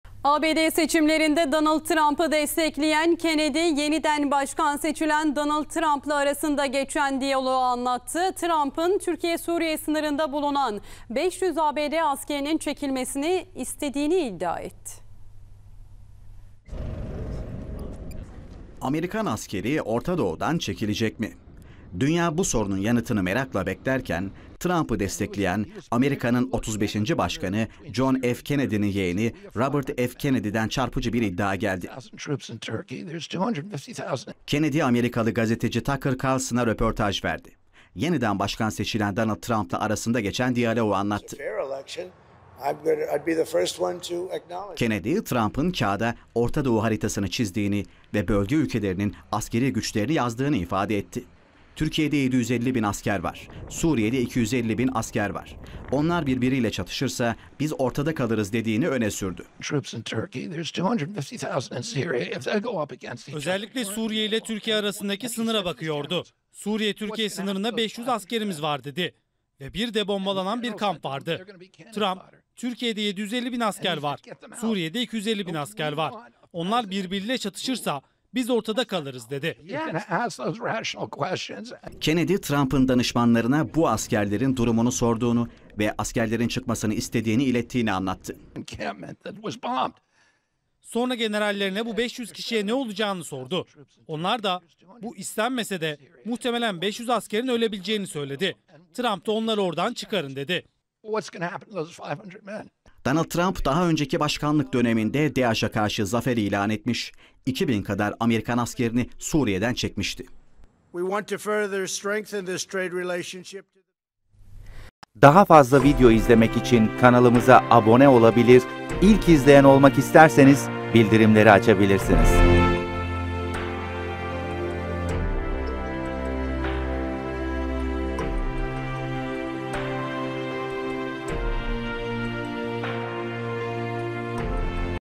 Trump İlk Hamlesini Suriye'ye Yapıyor
ABD Başkanı Trump, askerlerin Suriye'den çekilmesi için talimat verdi.

Türksat 4A
Frekans : 12.103
Sembol: 8333
Polarizasyon: Yatay ( Horz )
Modulasyon: 8SPK